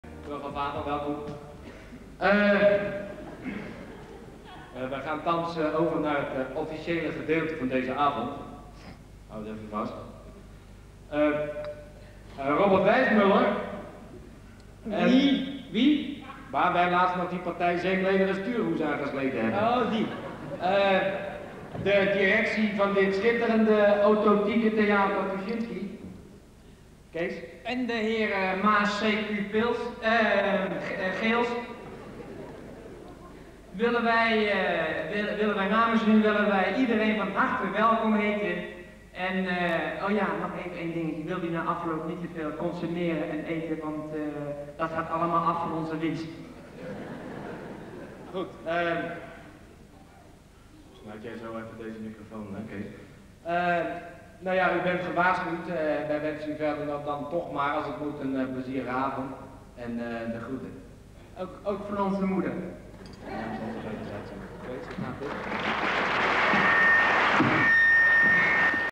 Flodder premiere Tuschinski theater (1986)
Huub Stapel en René van 't Hof houden een welkomstpraatje bij de premiere van Flodder in Tuschinski.